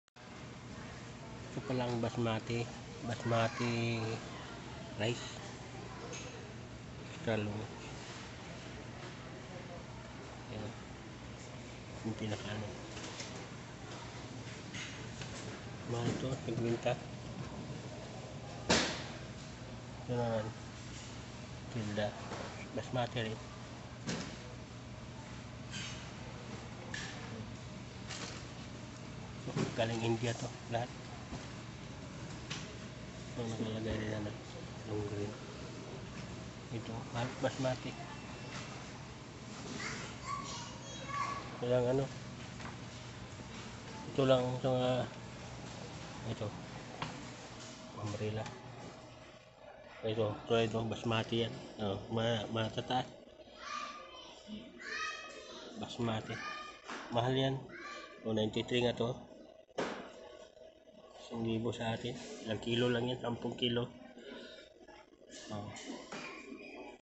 BASMATE RICE|| mayron na akong tanim Yan sa pilipinas galing India Dala Ng kaibigan ko si nafis
BASMATI RICE FROM INDIA